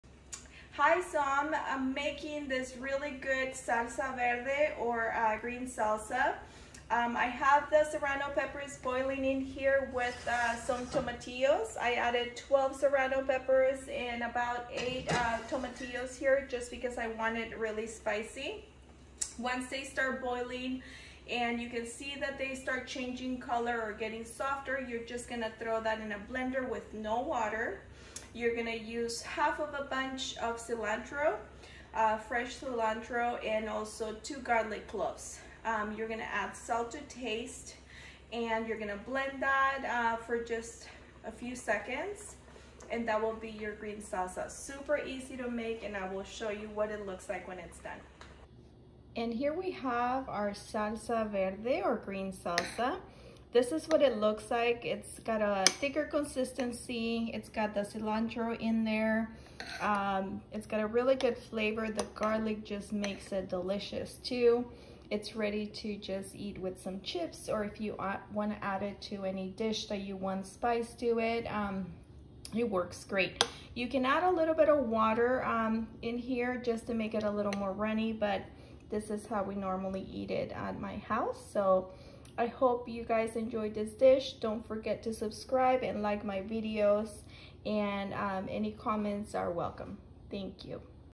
Salsa verde super easy to make! 😋😋Spicy 🌶🌶
Boiled 10-12 serrano prppers with 8 tomatillos for about 10-15 minutes or until they start changing color.
Drained the water throw in a blender with 1/2 of a cilantro bunch, two garlic cloves and salt to taste.
Blend for about 30 seconds and enjoy a delicious salsa verde or green salsa. You can eat with chips or add it to any of your dishes for the yummy spice. Gracias! 😘